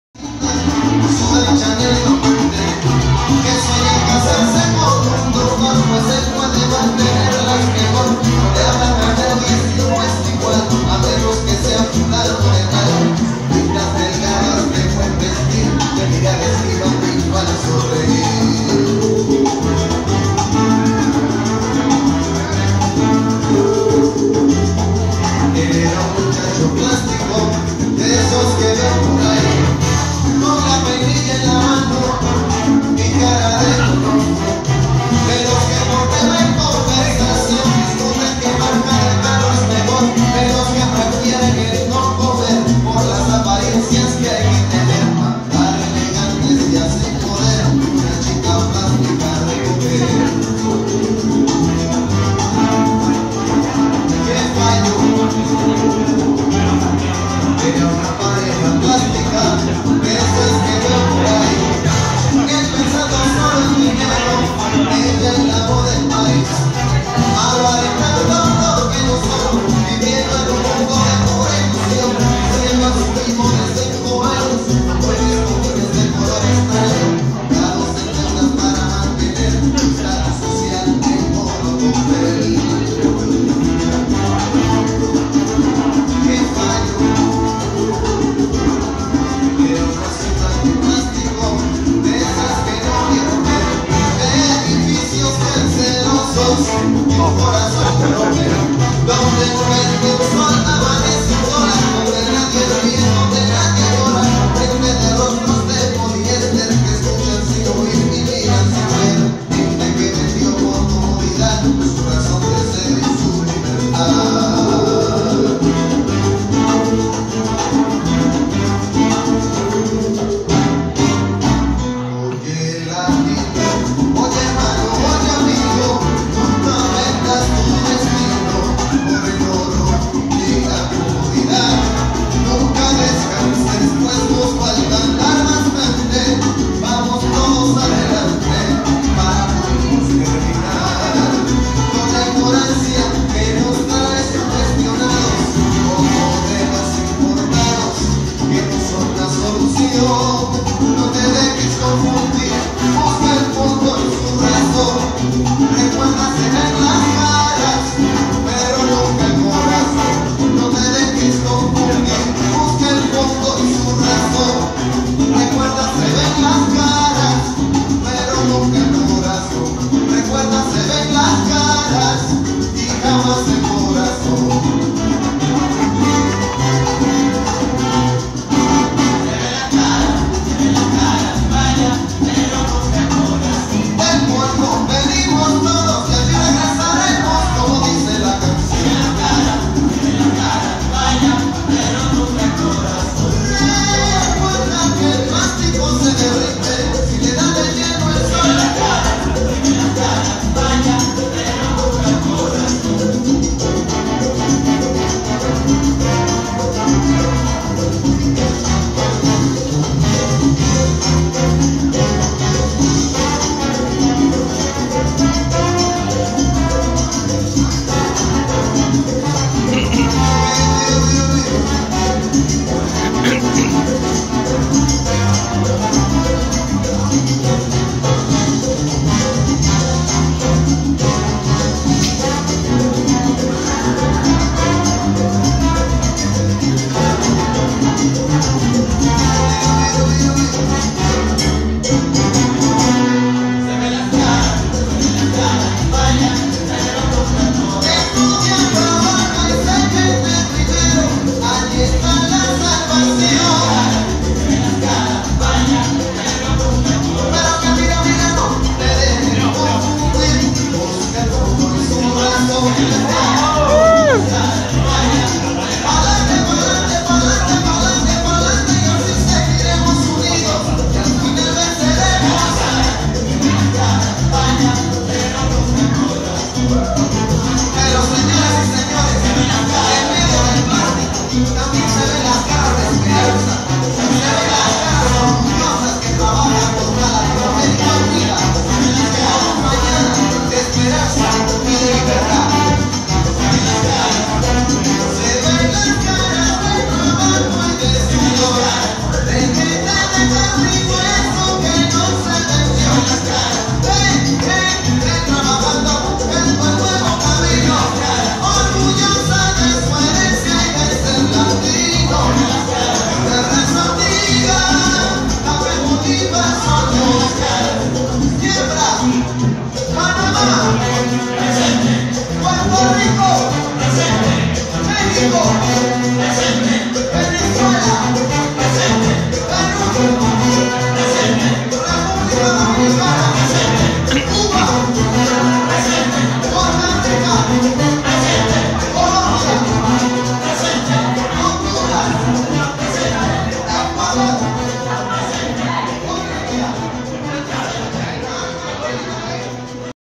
2016/9/2 Magna Gopal yi Omar salsa social@CopaTaiwan
Event: Magna Gopal in Taipei 2016 by Copa Taiwan
Instructor: Calvin Wang
DJ: Tienyu Tai / James Tsai
Venue: Asi Space / Taiwan